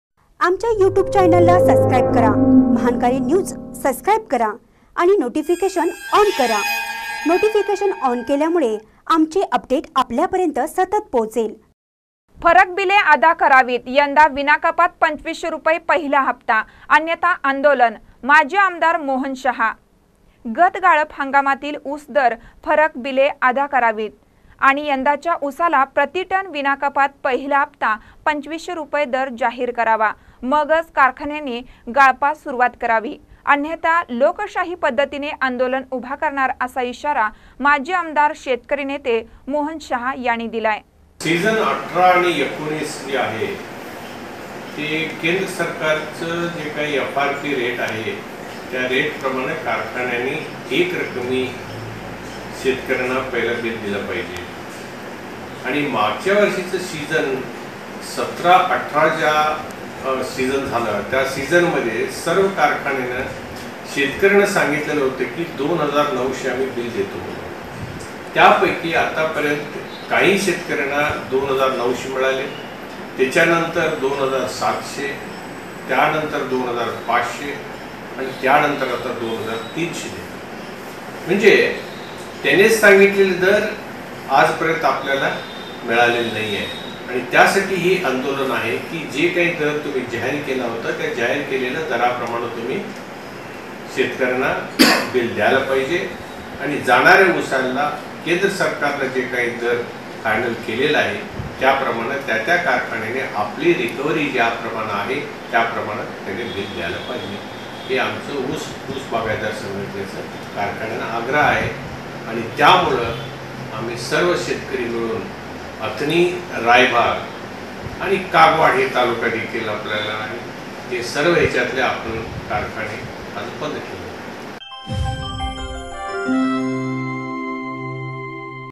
फरक बीले अदा करावीत, यंदा विनाकपात २५०० रु पहिला हप्ता, अन्यथा आंदोलन -
फरक बीले अदा करावीत, यंदा विनाकपात २५०० रु पहिला हप्ता, अन्यथा आंदोलन - माजी आमदार मोहन शहा